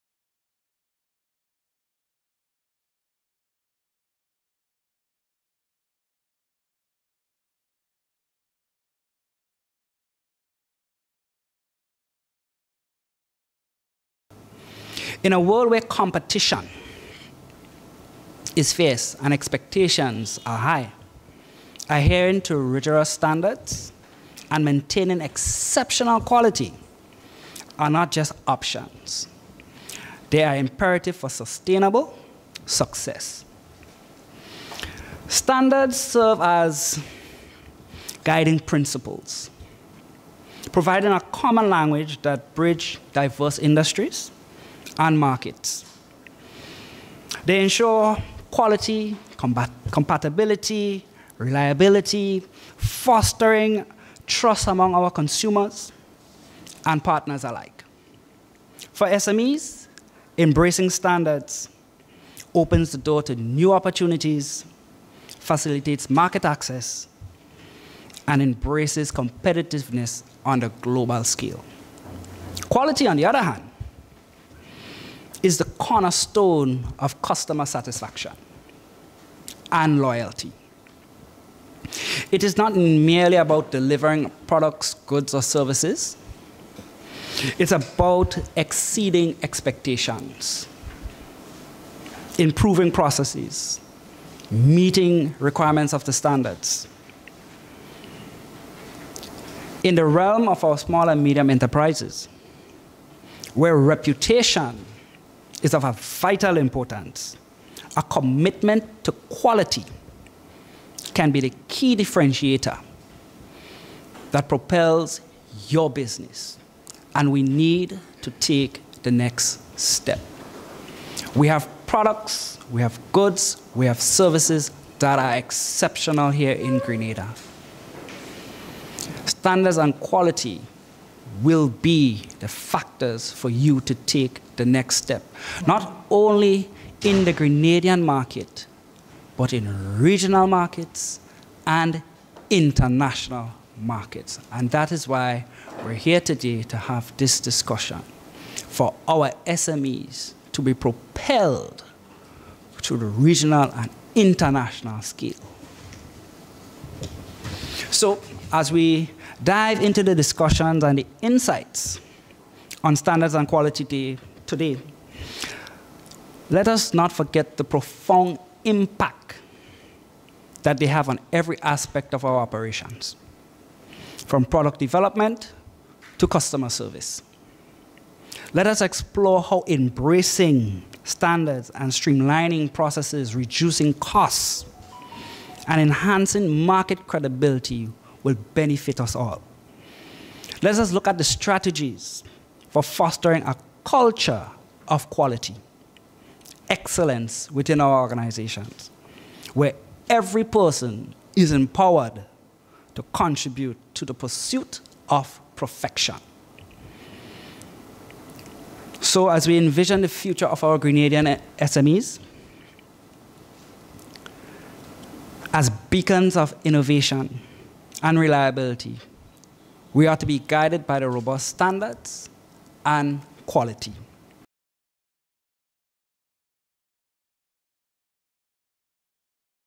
Robert Medford at the opening of Weeklong Workshop for SMEs
Several Grenadian agro-processors are among the participants of a one-week (Feb 19-23)  introductory training to quality management systems organized by the Grenada Bureau of Standards and the CARICOM Regional Organisation for Standards and Quality (CROSQ).

This week’s activity is allowing several Small and Medium Enterprises (SMEs) the opportunity to delve into the vital role of standards and quality, which opens the doors to new opportunities.

Robert Medford- Director of the Grenada Bureau of Stands, says this weeklong activity  will help participants including agro processors the opportunity to be guided by robust standards that facilitate market access and embrace competitiveness on a global scale.